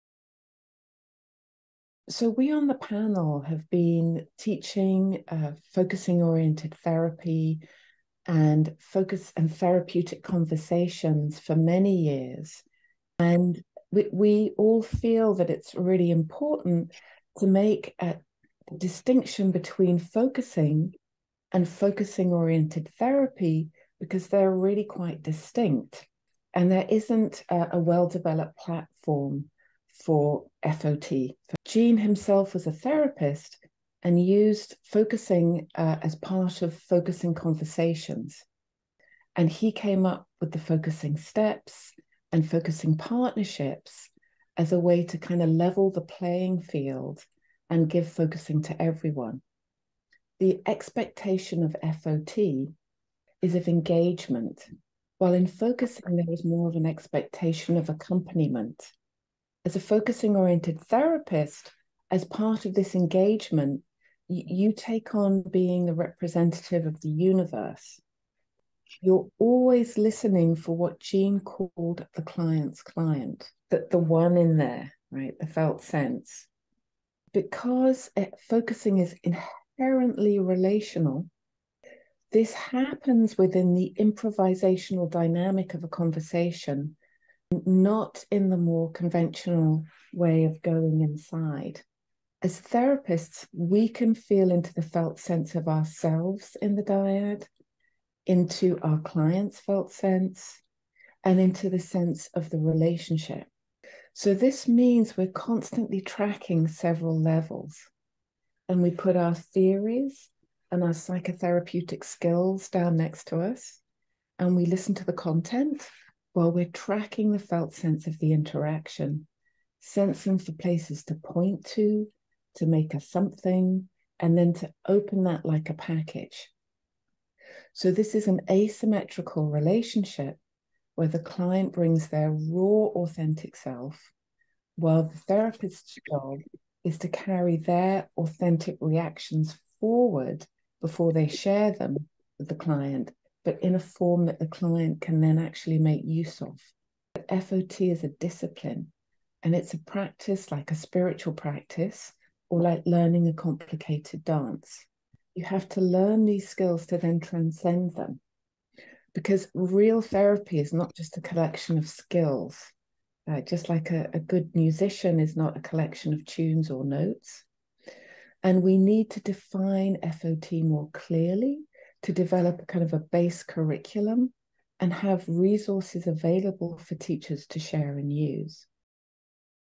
Charlotte Howorth's take on The Urgency of Teaching the Essence of FOT to a New Generation _ excerpt
A 3 Min Compilation of Charlotte Howorth's contribution to the Panel_ The Urgency of Teaching the Essence of FOT to a New Generation - FOT Conference 2023, with Biliania Dearly, Laury Rappaport, Charlotte Howorth, Joan Klagsbrun. Moderated by Jeffrey Morrison.